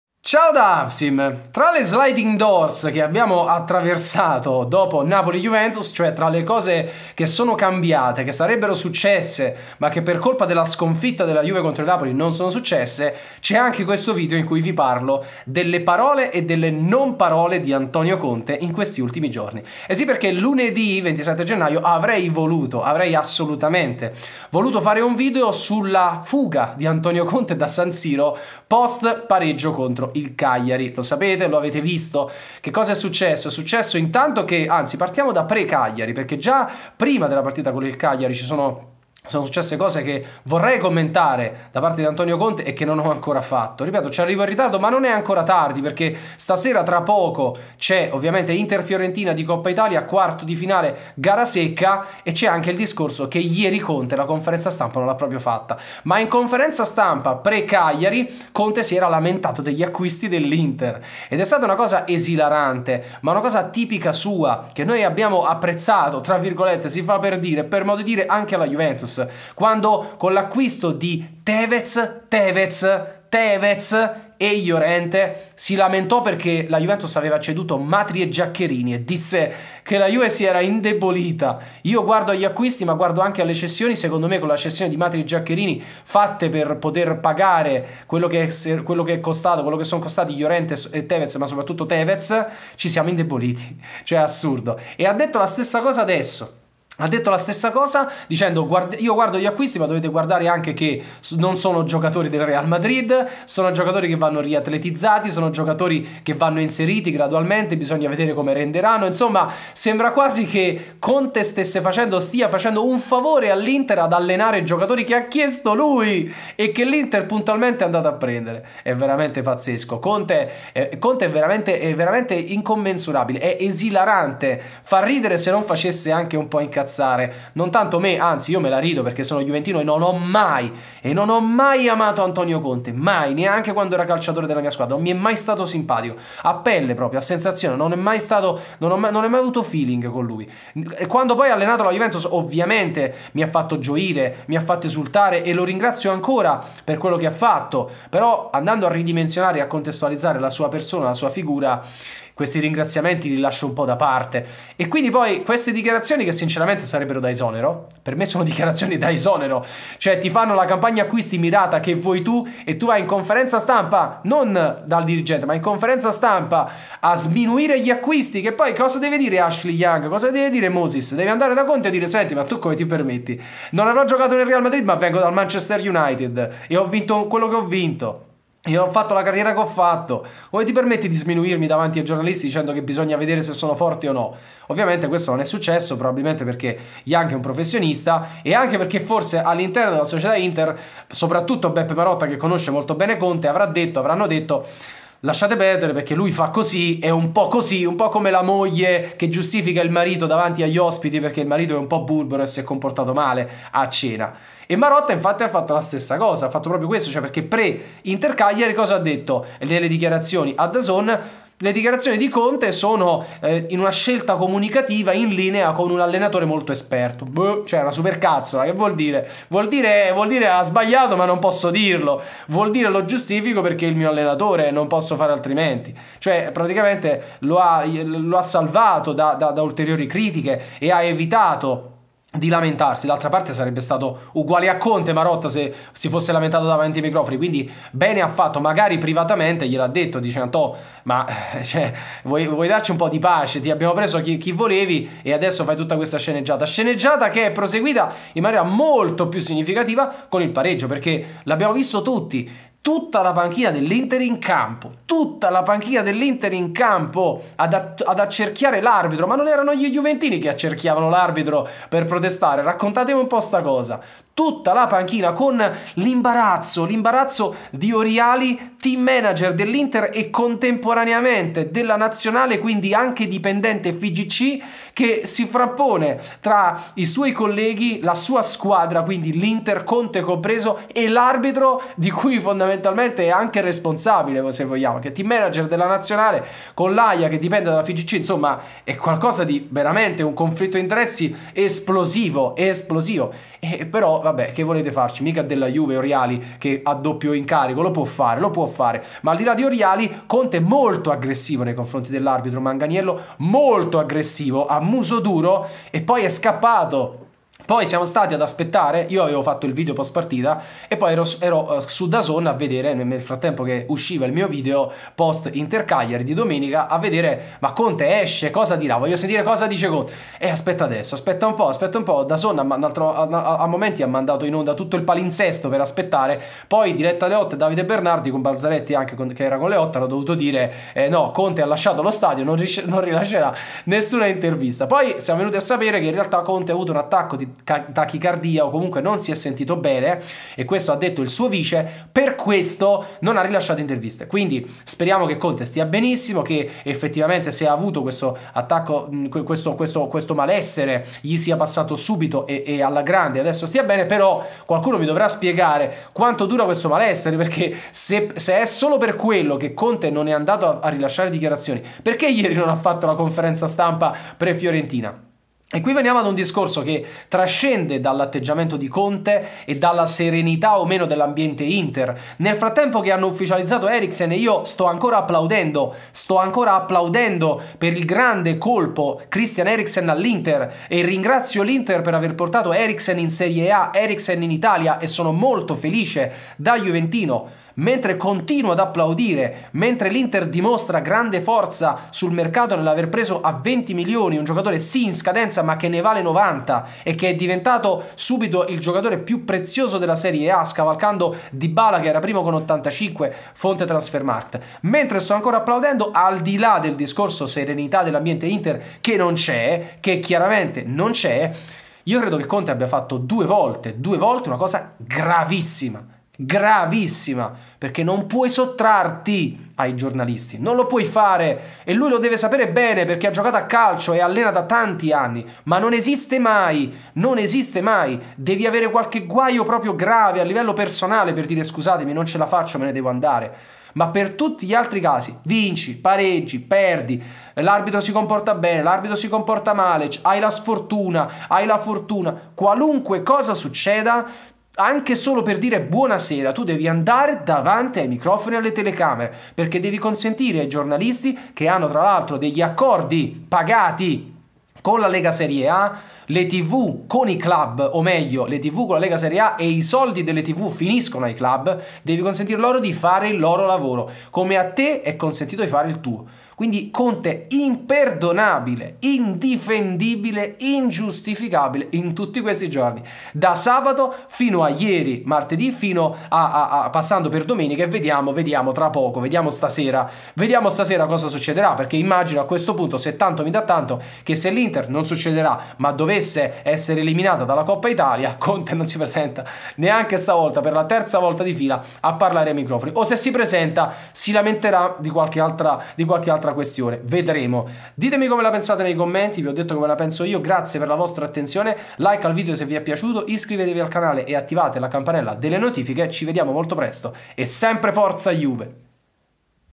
I piagnistei di Conte, irrispettoso verso i giornalisti ||| Extra Avsim
AntonioConte sta mostrando il peggio di sé. Prima, nella conferenza stampa pre InterCagliari, mostra plateale insoddisfazione verso il mercato fatto appositamente per lui dalla dirigenza dell'Inter, sminuendo i calciatori appena acquistati, poi non si presenta davanti ai microfoni di DAZN al termine della partita pareggiata proprio contro il Cagliari, infine annulla la conferenza stampa pre InterFiorentina di Coppa Italia mentre gli interisti possono gioire per l'arrivo di Eriksen. Conte serenissimo, insomma, per essere sarcastici. In realtà la cosa più grave è aver mancato di rispetto per ben due volte consecutive ai giornalisti.

SCARICA i miei STICKERS per WHATSAPP (si apre con l'app StickerMaker) Mostra la tua gratitudine per la mia attività: urly.it/3zv4h